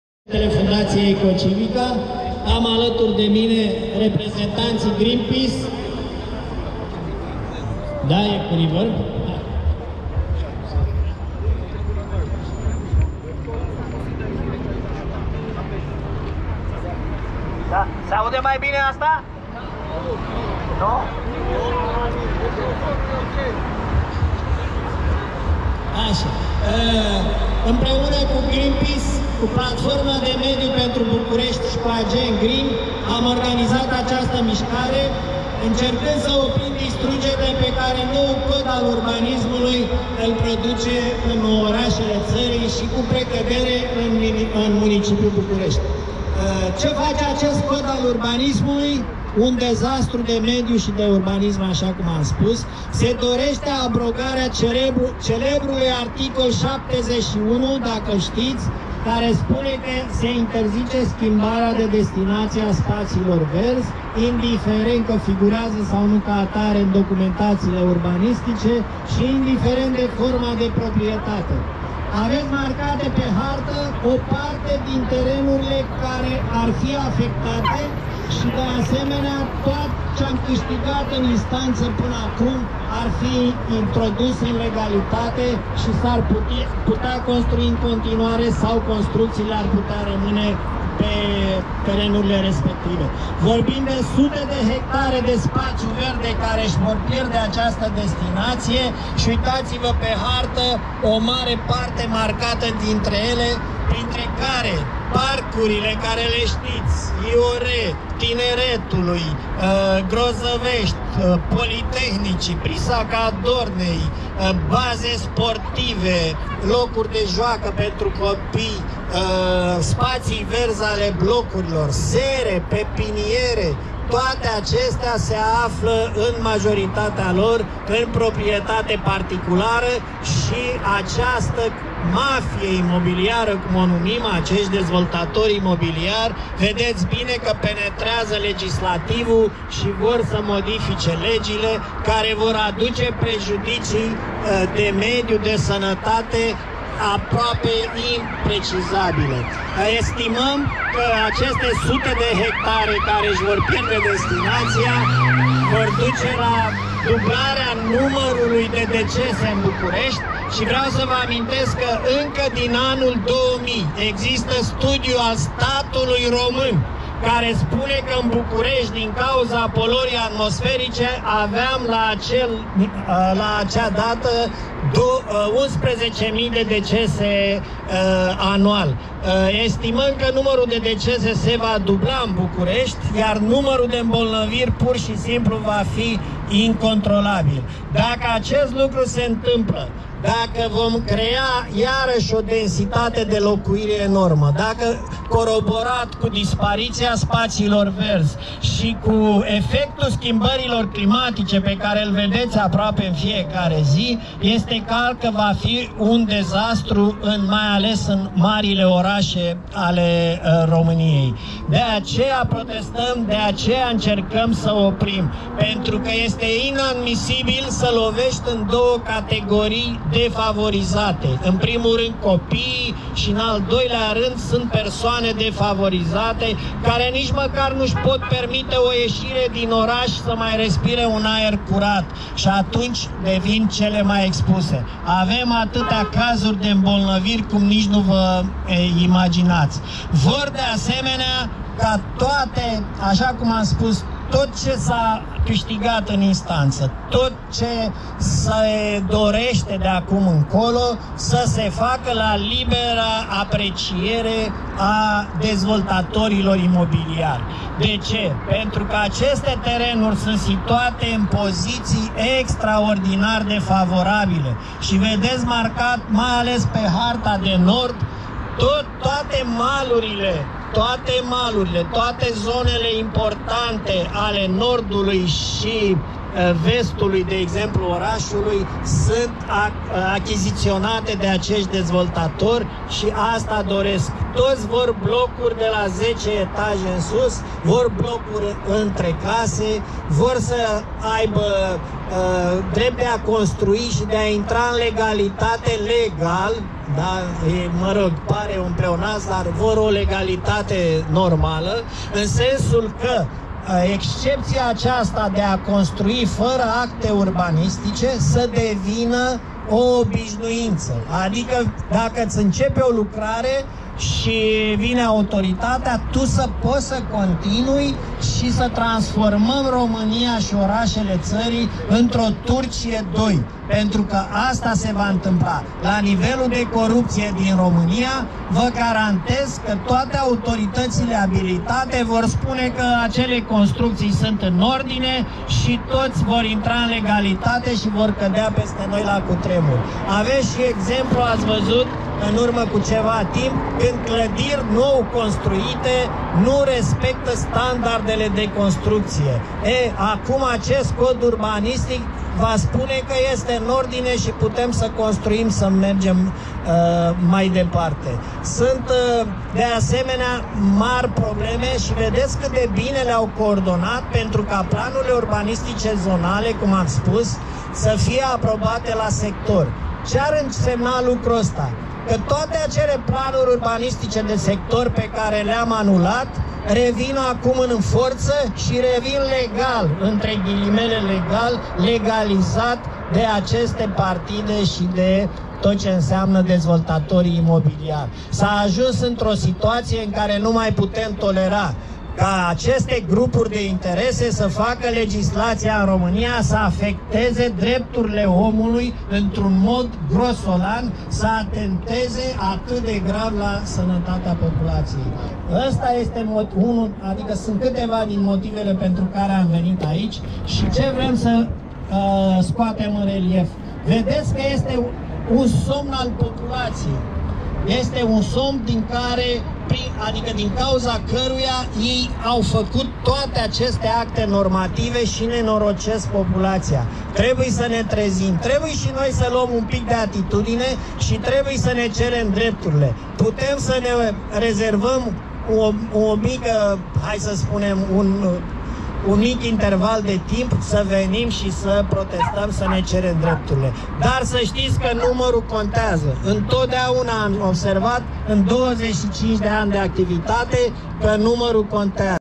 Protest în Piața Victoriei contra amendamentelor mafiei imobiliare
La invitația fundației Eco-Civica și GreenPeace am ieșit azi la protest în fața Guvernului împotriva unor inițiative legislative care riscă să sufoce capitala și alte orașe mari